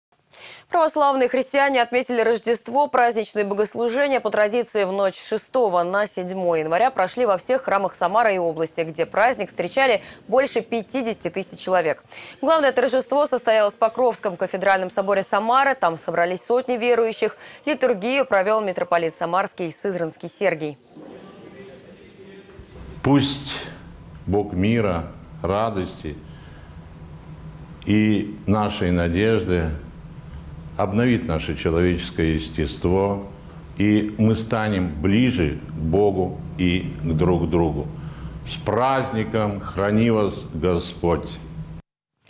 Православные христиане отметили Рождество
Праздничные богослужения по традиции, в ночь с 6 на 7 января, прошли во всех храмах Самары и области, где праздник встречали больше 50 тысяч верующих. Главное торжество состоялось в Покровском кафедральном соборе Самары. Там собрались сотни верующих. Литургию провел митрополит Самарский и Сызранский Сергий.